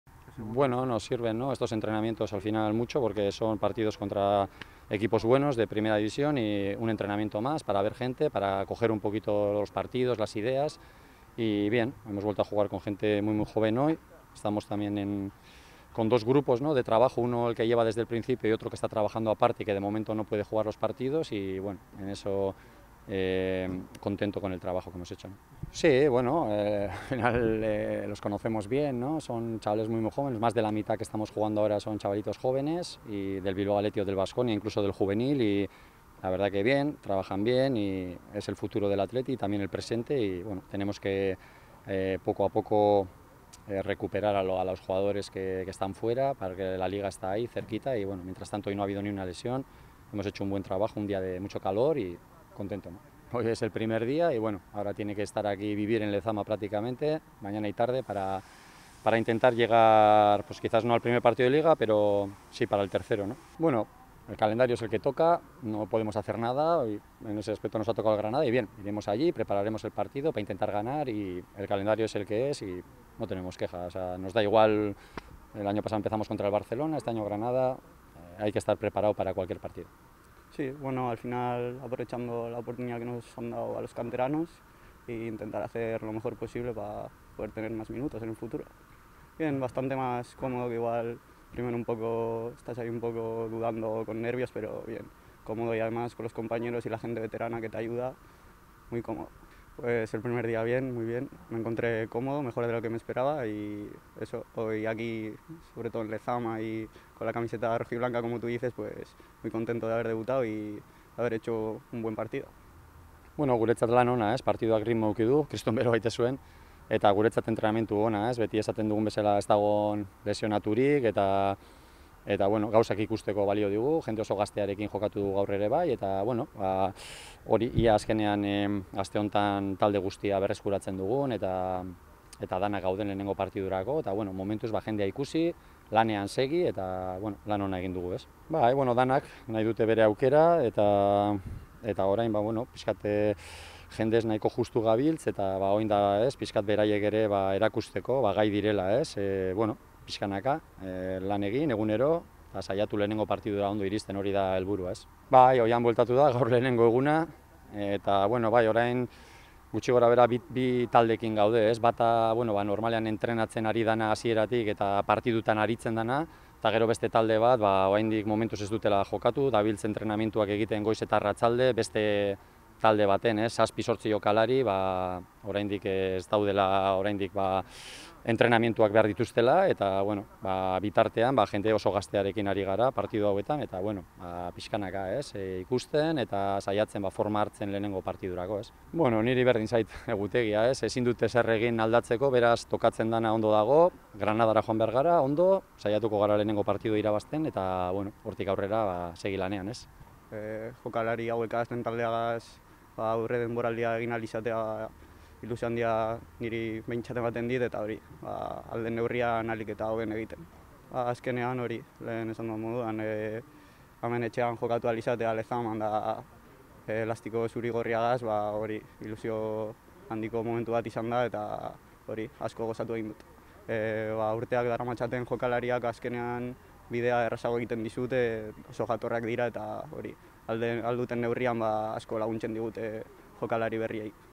🎙️ Gaizka Garitano & Oier Zarraga - post Athletic Club - Deportivo Alavés
Declaraciones de Gaizka Garitano y Oier Zarraga, entrenador y jugador del Athletic Club, tras la victoria en Lezama por 2 goles a 1 frente al Deportivo Alavés.

Gaizka Garitano eta Oier Zarraga, Athleticeko entrenatzaile eta jokalariaren adierazpenak, Lezaman Deportivo Alavesen aurka 2 eta 1 irabazi ostean.